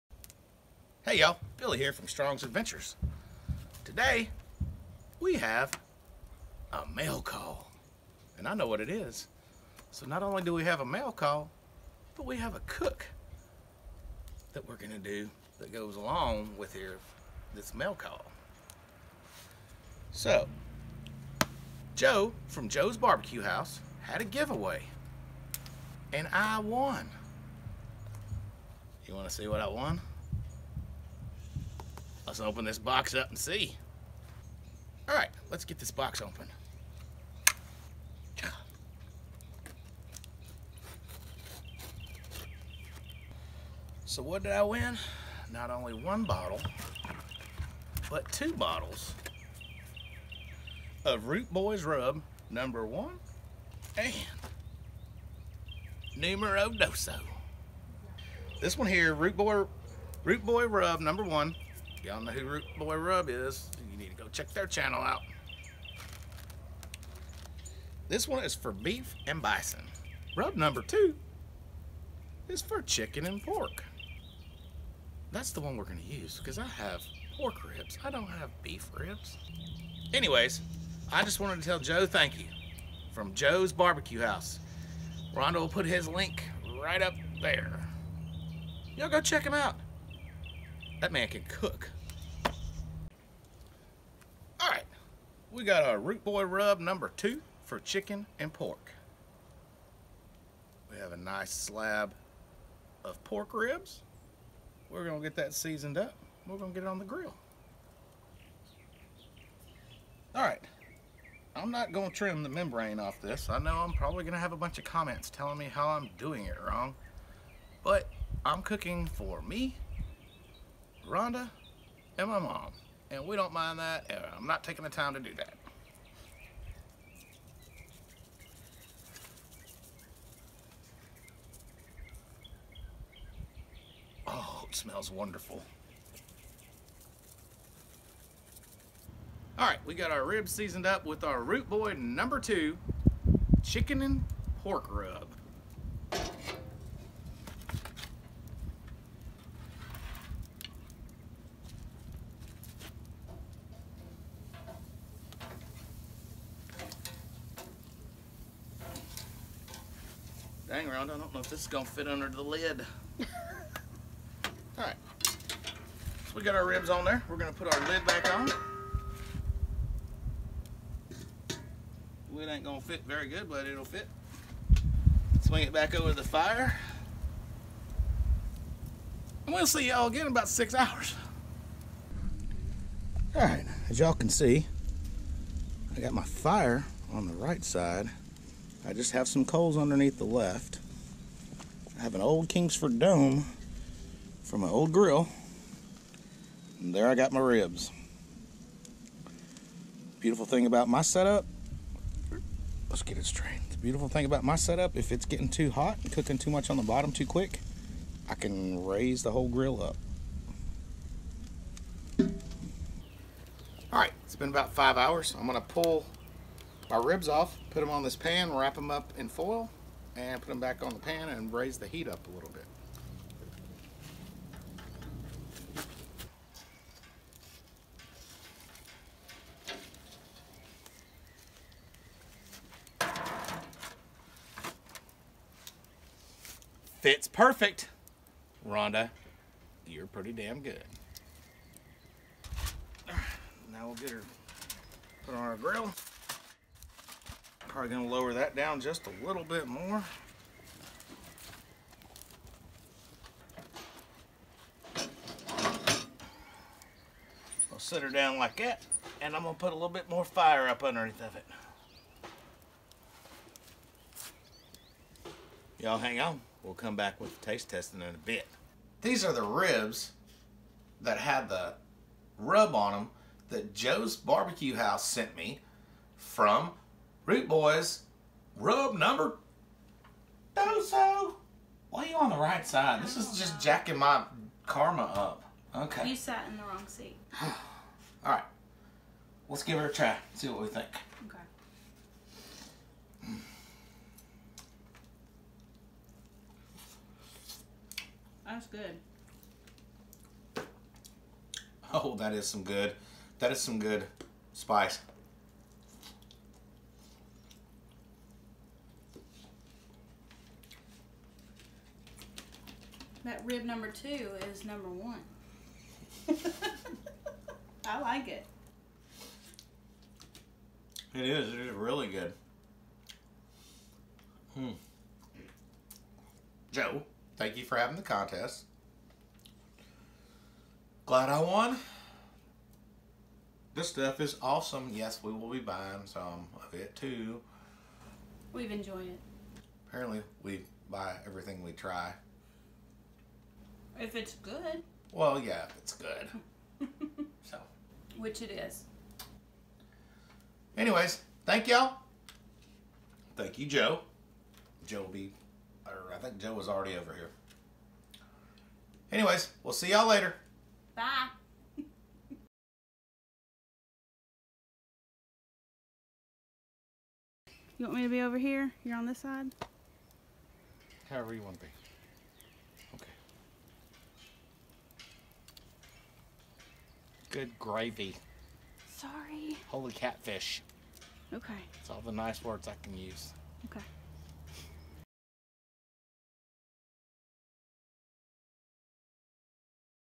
Smoked Ribs on the Swing Grill - Mail Call
We smoked some ribs on the outdoor cooking fire box with swing grills.  Billy won a give away from Joe's BBQ House.  It was 2 different rubs from The Root Boy Cooks!  Their links are below...

Or you can send us stuff at